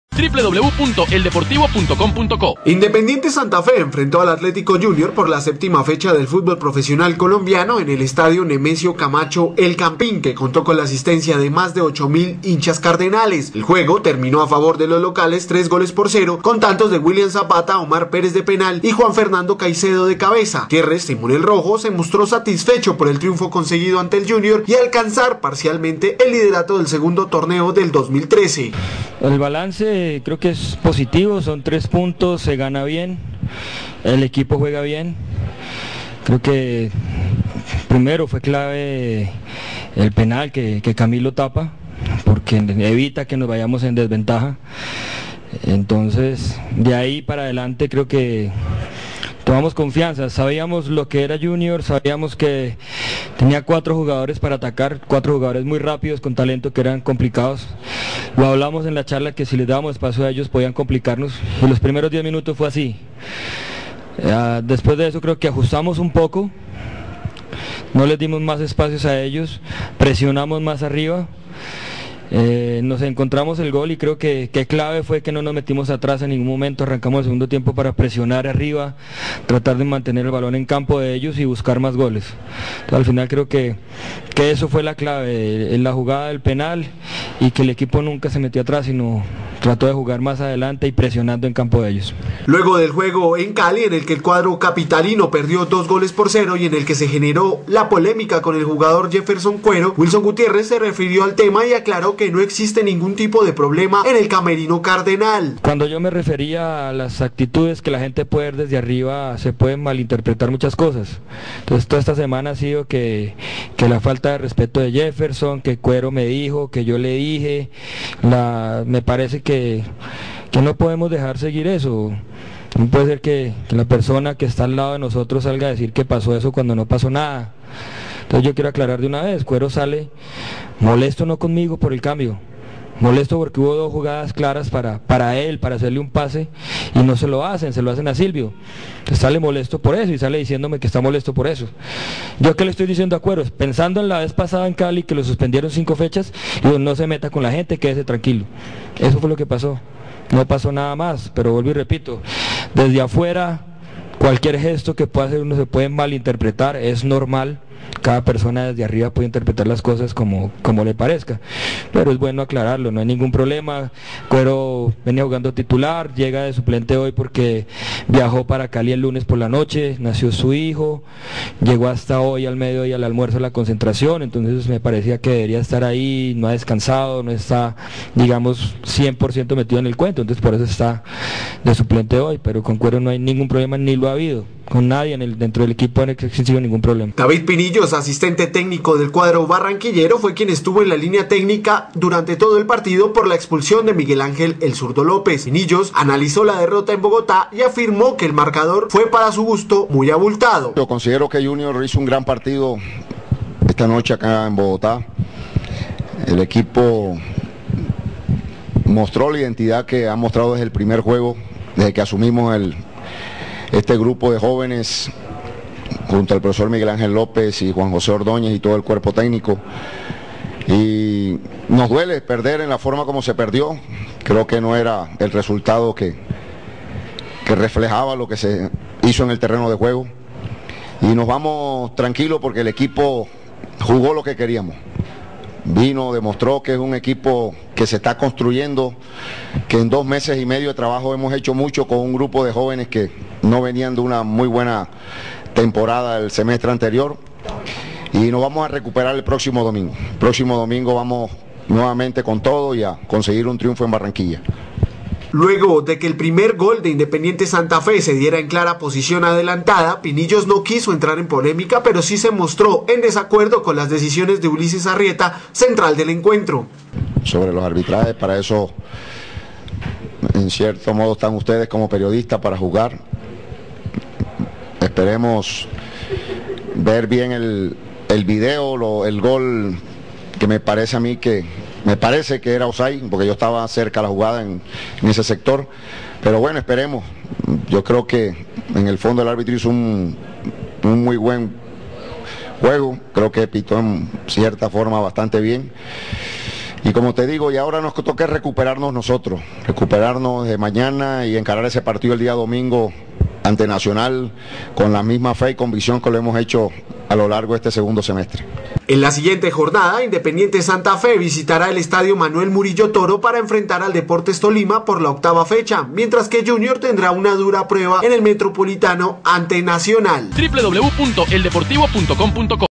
Santa Fe vs Junior (Resumen)
"Fue clave la atajada del penal": Wilson Gutiérrez

Independiente Santa Fe enfrentó al Atlético Junior por la séptima fecha del FPC en el estadio Nemesio Camacho "El Campín" que contó con la asistencia de más de 8 mil hinchas Cardenales. El juego terminó a favor de los locales 3-0 con tantos de William Zapata, Omar Pérez de Penal y Juan Fernando Caicedo de cabeza.